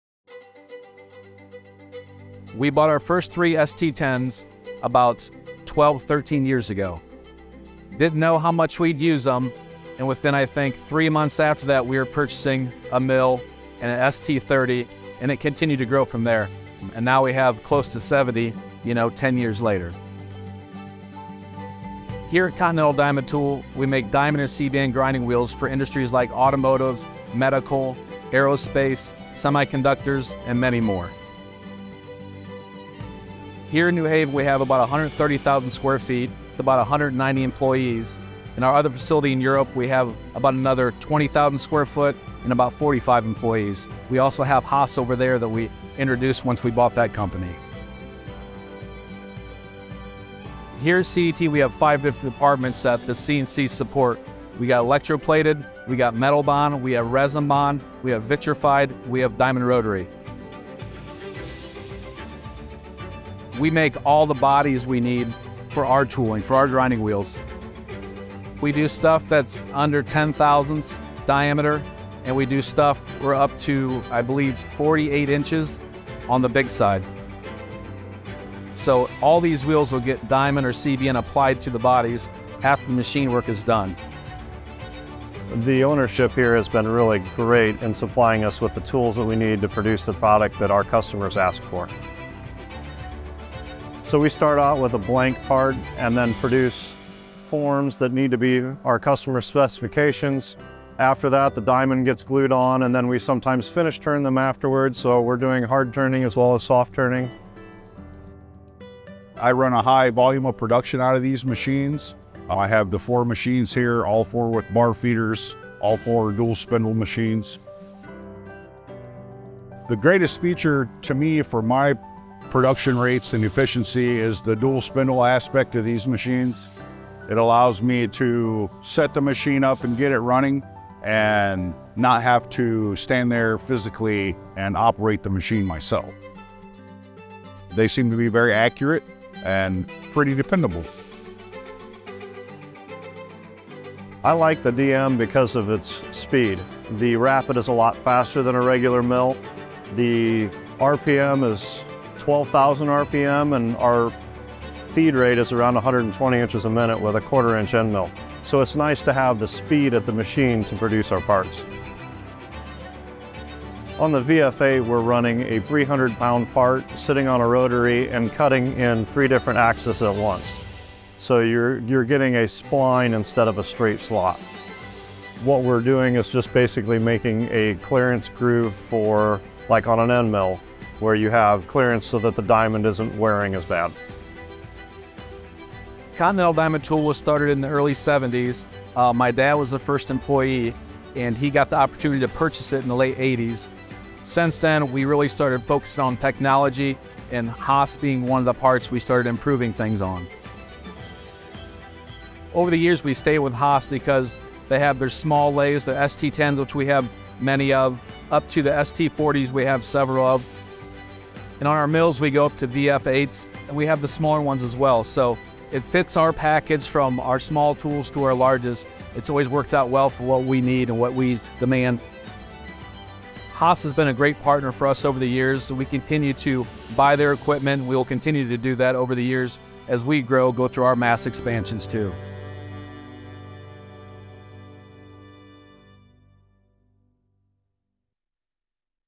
Machining Grinding Wheel Bodies at Continental Diamond Tool (CDT) on Haas
Continental Diamond Tool (CDT) purchased their first CNC lathes, (3) Haas ST-10's over 12 years ago and today operates over 70 Haas CNC lathes and mills. In their machine shop, they machine the bodies for their popular range of diamond and CBN grinding wheels, used in a wide range of industries that include automotive, medical, aerospace, semiconductors, and more.

Many of their Haas CNC lathes are automated with bar feeders and robots and feature dual-spindles or sub-spindles to machine parts complete. They also have several Haas vertical machining centers, ranging from a DM-1 compact drill/mill centers all the way up to VF-8 large capacity machining centers with 4th axis.

Enjoy this Haas INSIDER video with CDT owner Nick Viggiano, CNC Department Foreman, Josh Werling and Lead CNC Lathe Operator Ron Fuller.